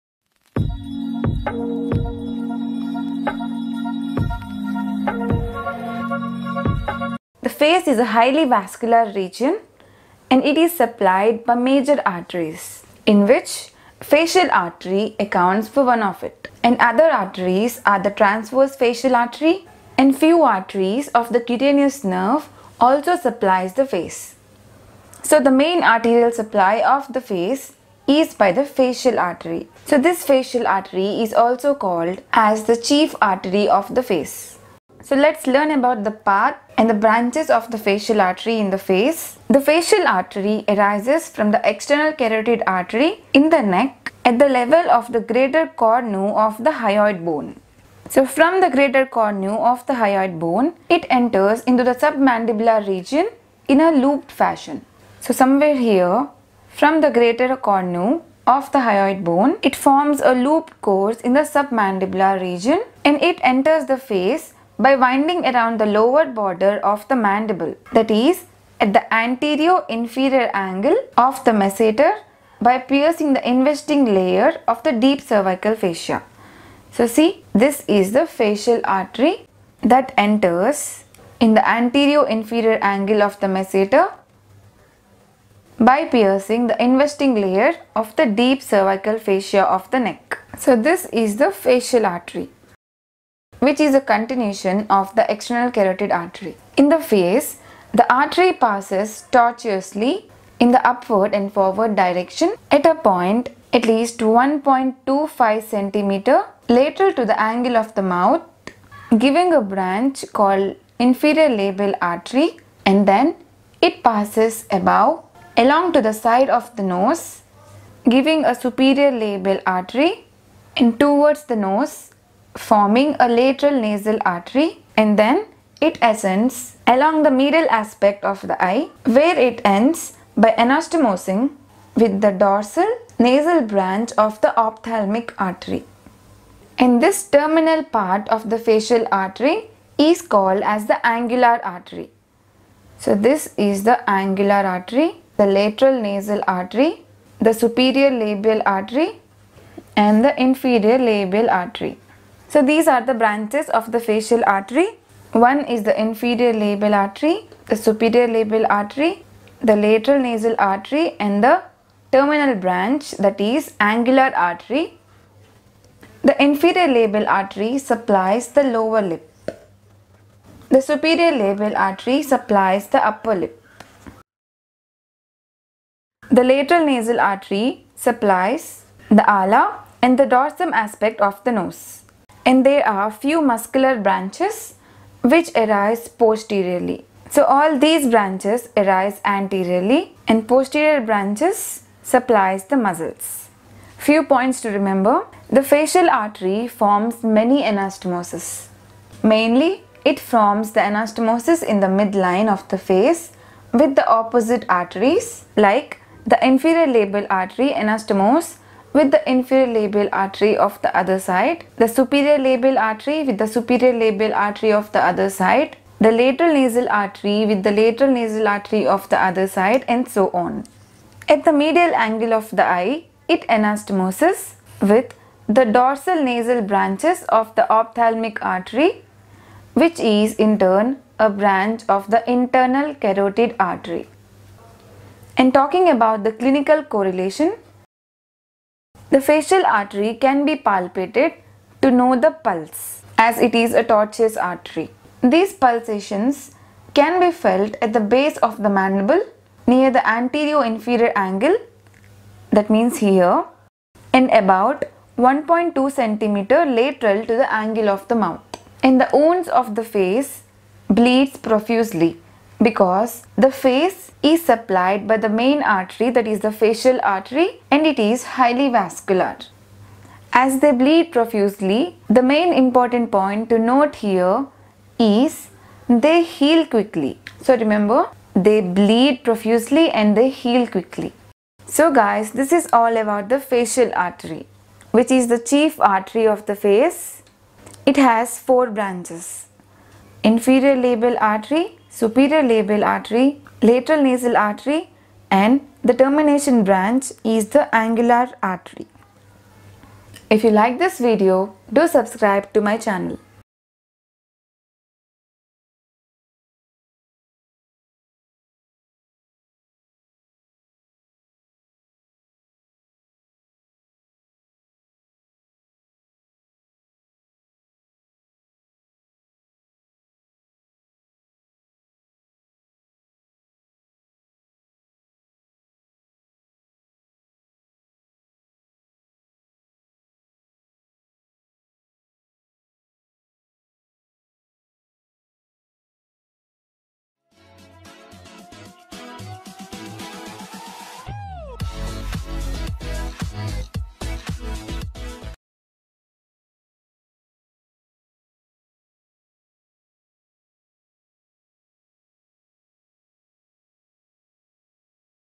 Facial Artery | Anatomy | Origin | Branches | Anastomosis | Knowing Anatomy
Source External carotid artery
Branches Cervical branches:
- Ascending palatine
- Tonsillar
- Submental
- Glandular

Facial branches:
- Inferior labial
- Superior labial
- Lateral nasal
- Angular

Mnemonic: Anna, PLS SIT (Ascending palatine artery, Premasseteric artery, Lateral nasal artery, Submental artery, Superior labial artery, Inferior labial artery, Tonsillar artery)
Supplies Muscles and skin of the face The facial artery is a branch of the external carotid artery that supplies the anatomic structures of the superficial face. The facial artery arises from the carotid triangle which is formed by the superior belly of the omohyoid muscle, the sternocleidomastoid, and the posterior belly of the digastric muscle.